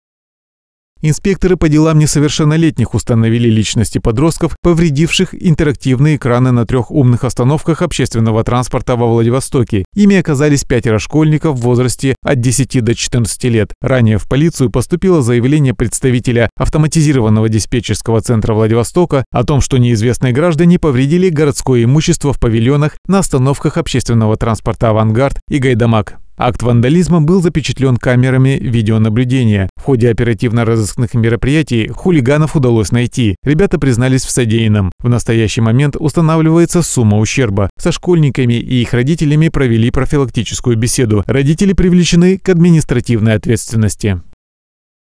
Полиция Владивостока разыскала подростков, повредивших интерактивные экраны.
Инспекторы по делам несовершеннолетних установили личности подростков, повредивших интерактивные экраны в трёх «умных» остановках общественного транспорта во Владивостоке. Ими оказались пятеро школьников в возрасте от 10 до 14 лет.